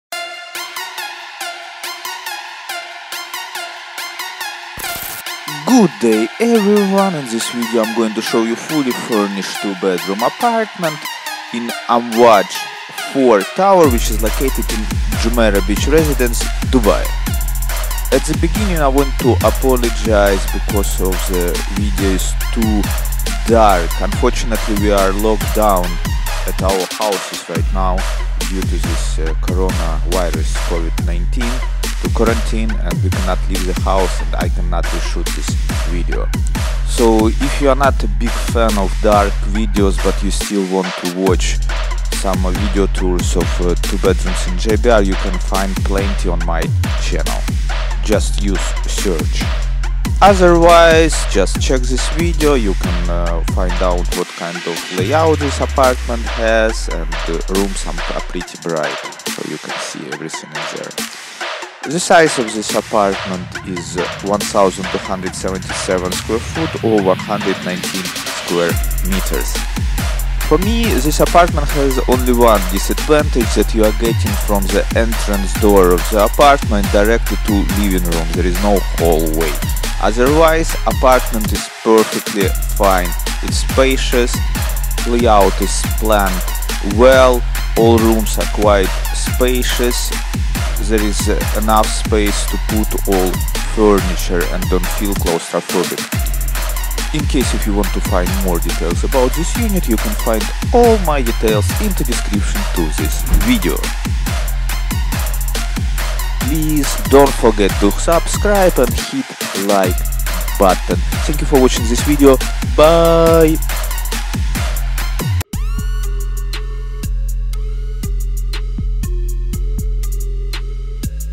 Bigger size 2 bedroom apartment in Amwaj 4 in JBR
Fully Furnished 2 bedroom apartment
Project: Jumeirah Beach Residences (JBR)
Building: Amwaj 4
Bedroom: Two
Bathroom: Three
Balconies: One
Kitchen: Equipped
Parking: One basement
Views: Marina View
Tenancy: Vacant
Size: 1277 sq.ft. (119 sq.mt.)

Jumeirah Beach Residence (also known as JBR) is a 1.7 kilometres (1.1 mi) long, 2 square kilometres (0.77 sq mi) gross floor area waterfront community located against the Persian Gulf in Dubai Marina in Dubai, United Arab Emirates. It is the largest single phase residential development in the world and contains 40 towers (36 are residential and 4 are hotels). JBR can accommodate about 15,000 people, living in its apartments and hotel rooms. The Project has 6,917 apartments, from 900 sq ft (84 m2) studios to 5,500 sq ft (510 m2) penthouses.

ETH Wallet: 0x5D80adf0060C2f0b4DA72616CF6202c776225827
BTC Wallet: 3C1HNYr9XQ7tNKioFeiLhFro9LJdWtZQ2G